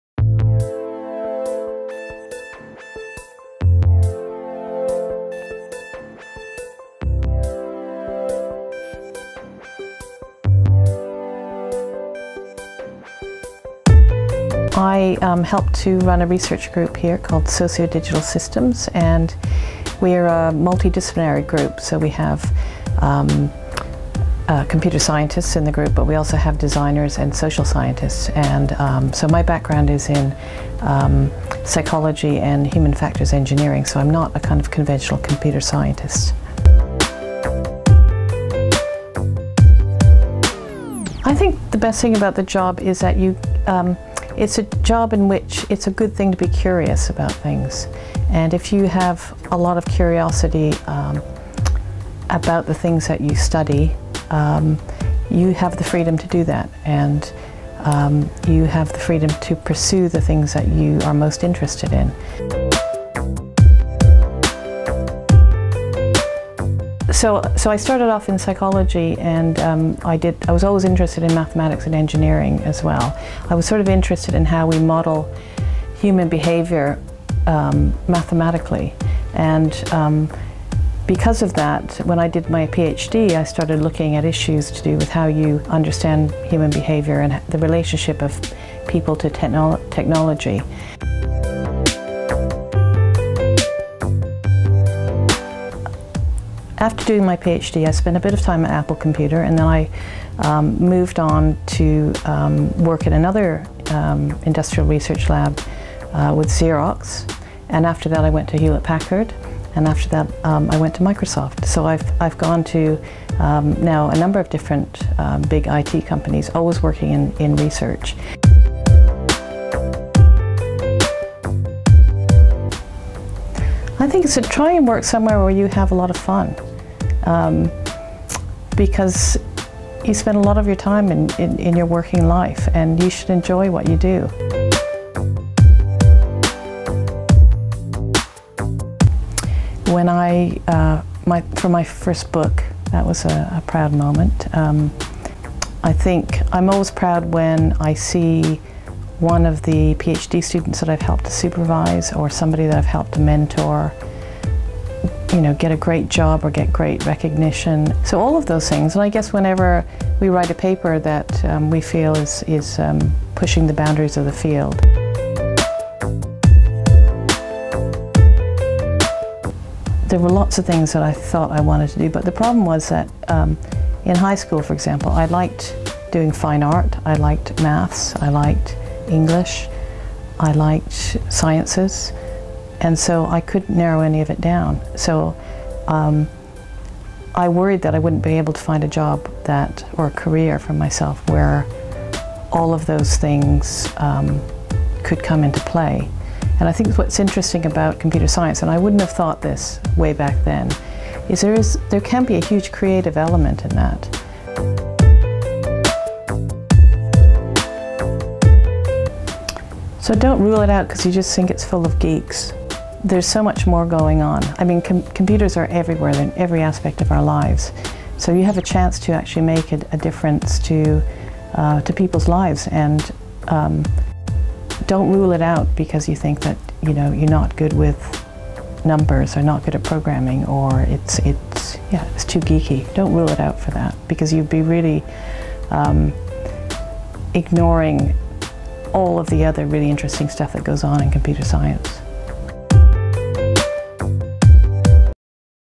Microsoft Research - Women in Computer Science: Abigail Sellen
Principal Researcher Abigail Sellen talks about her career and role at Microsoft Research.